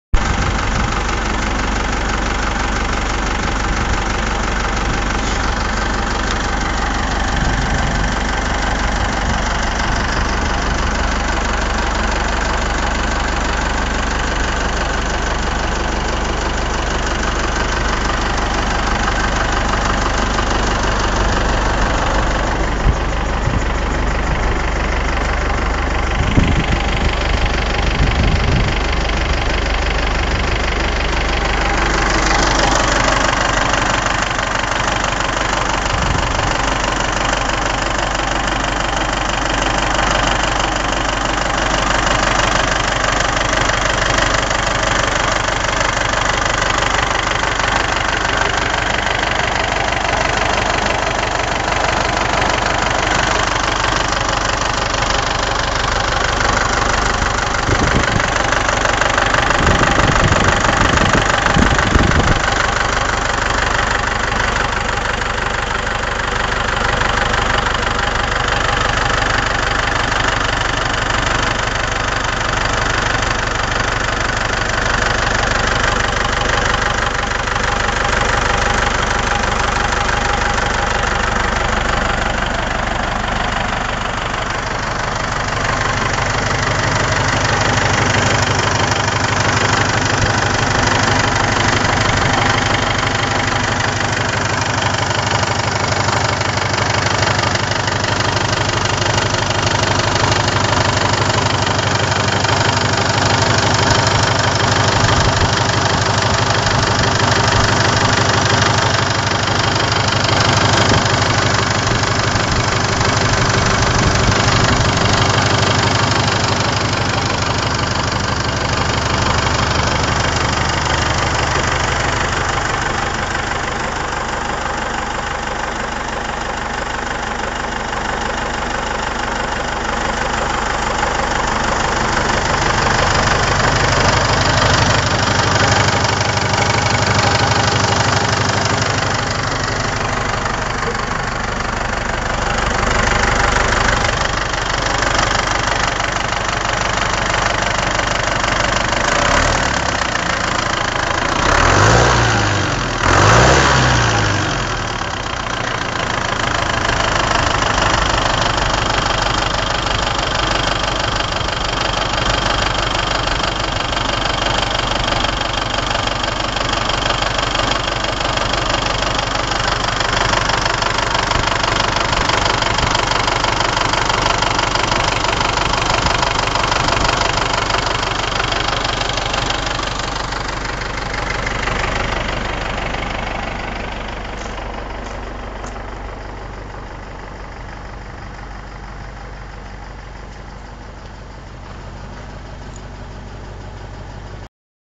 MB Trac 800 with OM364A
Our trac with the OM364A mounted and running. For histiory check forum at trac-technik.de